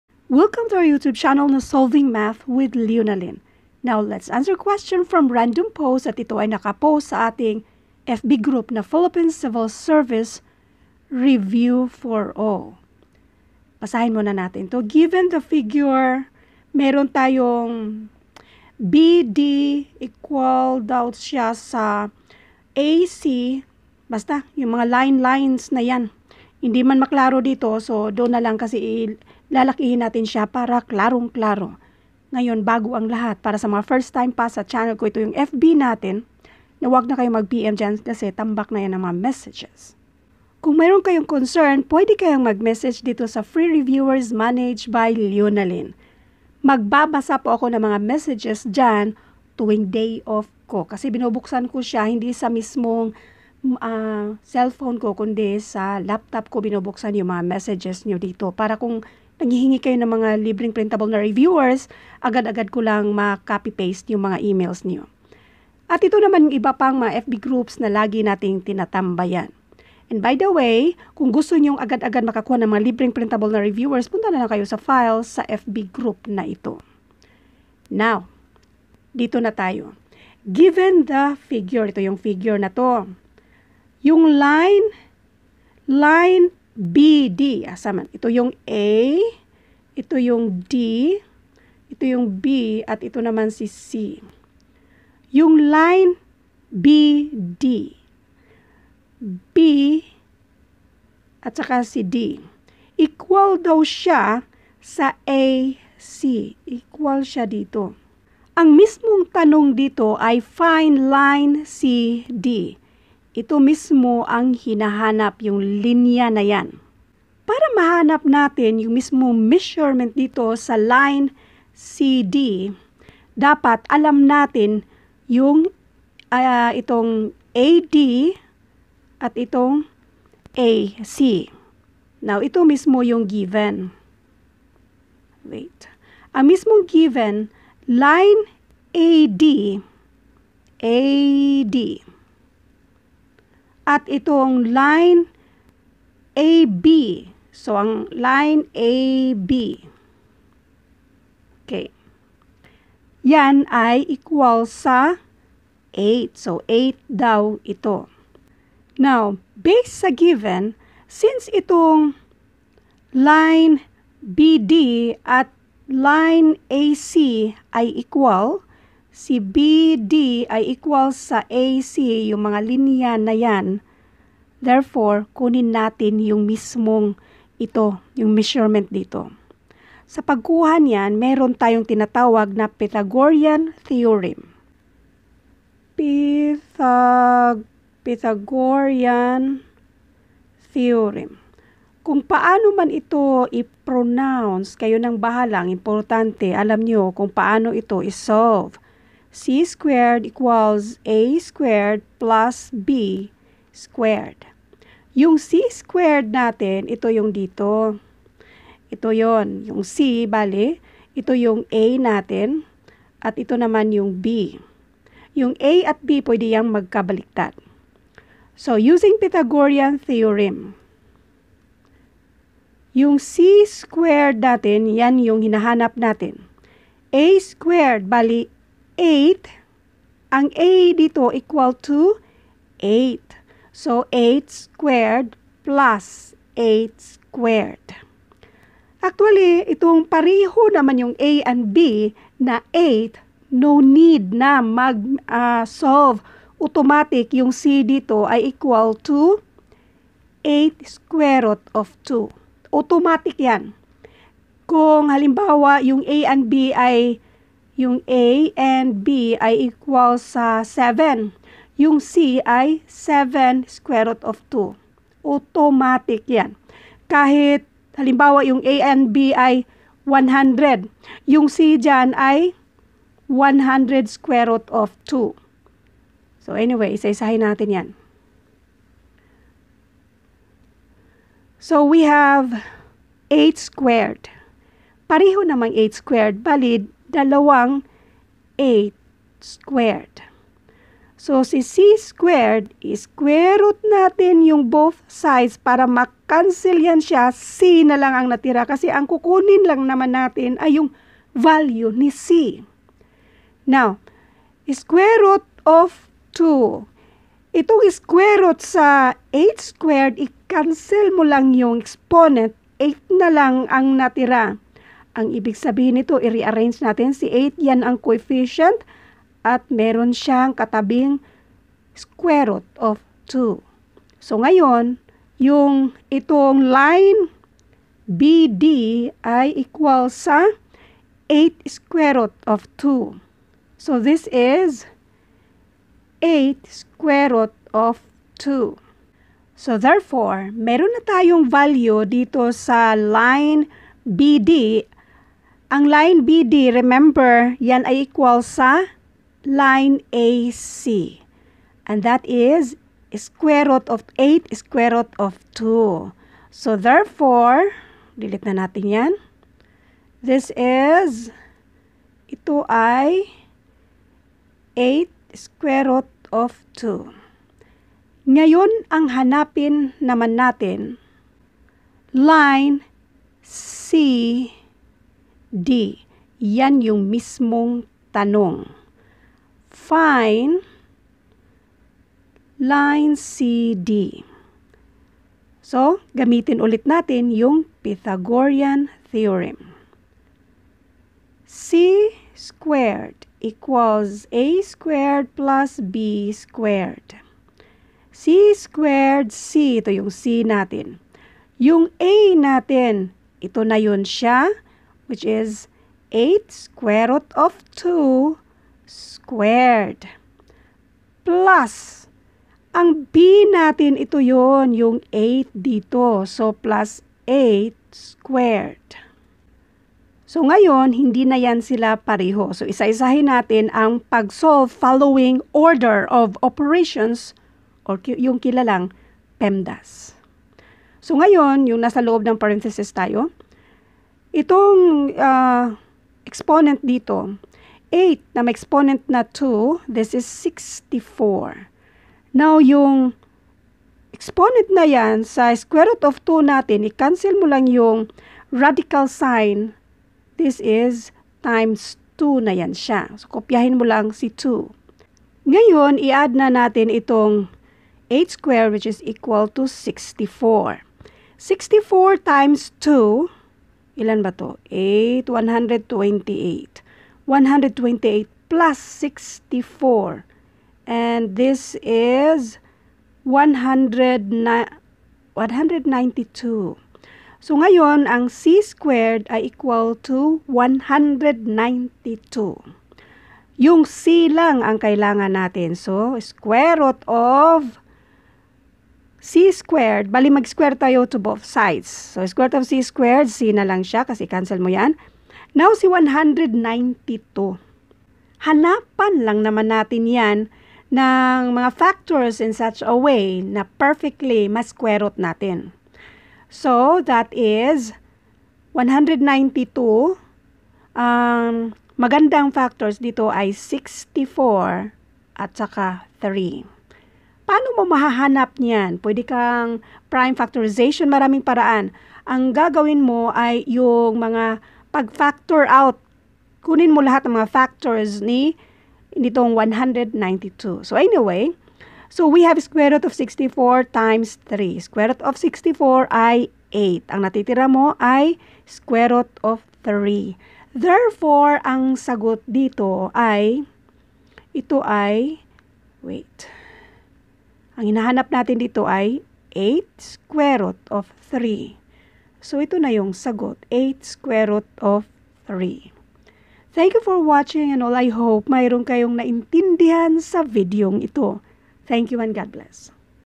Given the figure line BD = AC, line AD = AB = 8 Find line CD | Geometry Pythagorean Theorem
choices are
a. 8√2
b. 8√3

Free Civil Service Reviewers managed by Leonalyn

Philippine Civil Service Review for all

Editing Apps: VivaVideo
SamsungNote5: Snote
Back-up: SamsungNote9